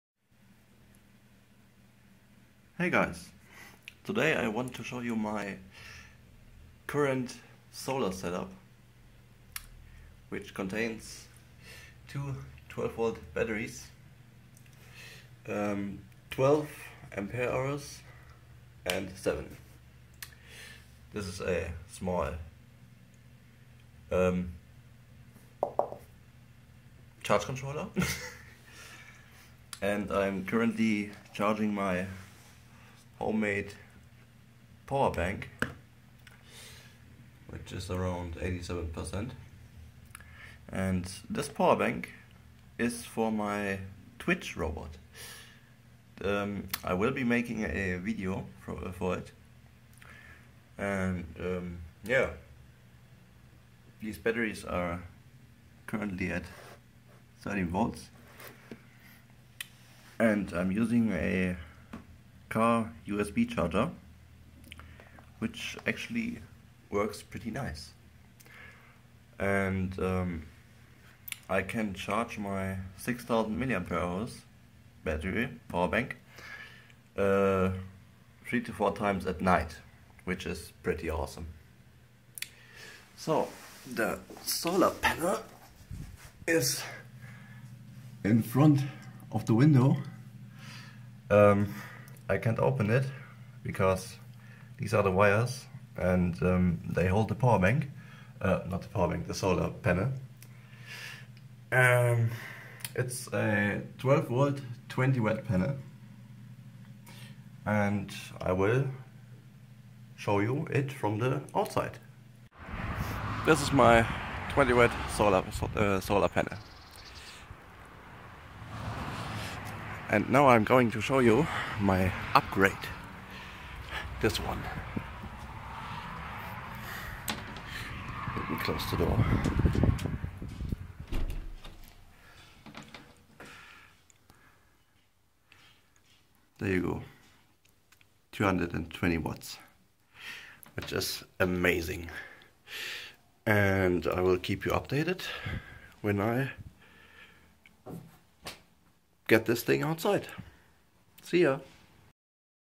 My Solar System (Currently 20 Watt)
(So i just uploaded the wrong video :D) This is my homemade Solar System, it will get upgraded in the future. I use it to charge my powerbanks and my smartphohne.